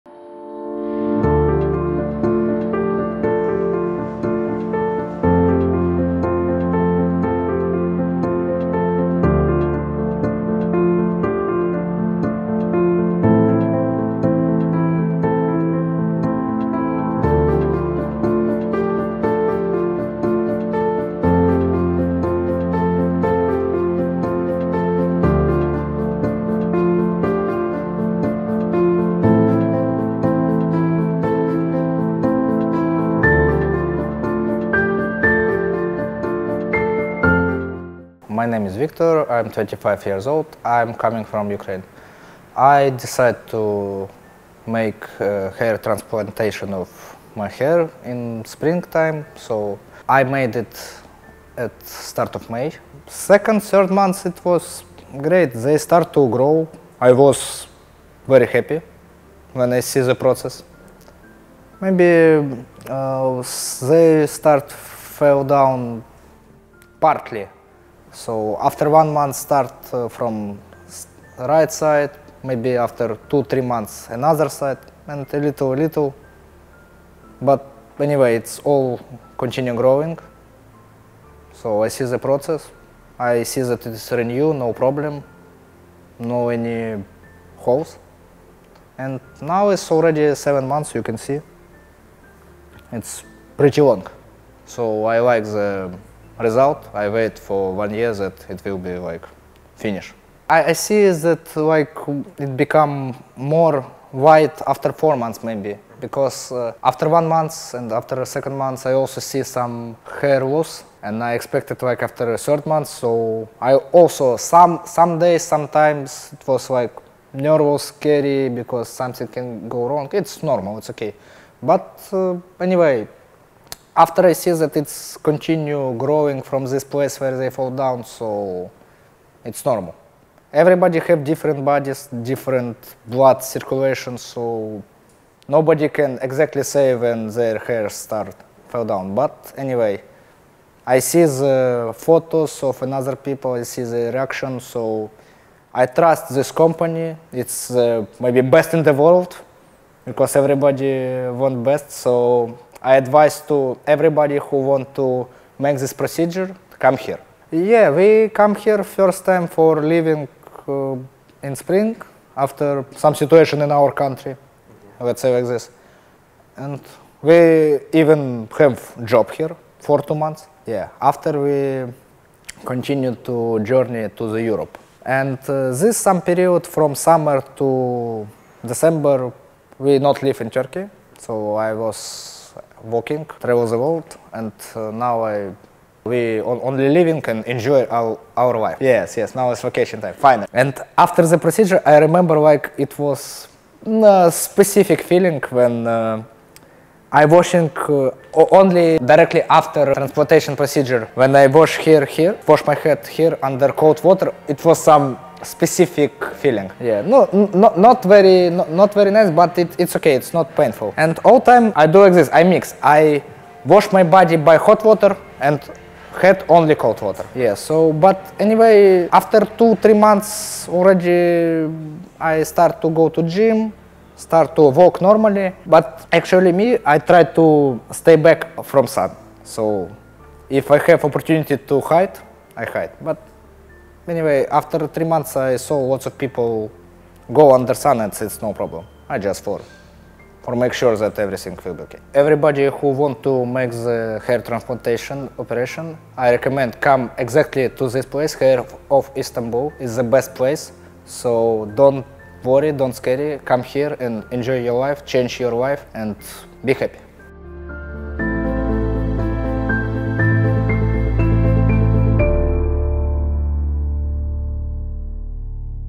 Before and After Hair Transplant
Victor is from Ukraine and is 35 years old. He explains the different stages of hair growth after the hair transplant. He says that he is happy with his results. He tells about his journey to Turkey and the reason he came here. He tells about his experience after the procedure. He recommends Hair of Istanbul to every person who wants to get a hair transplant.

Shorts ↴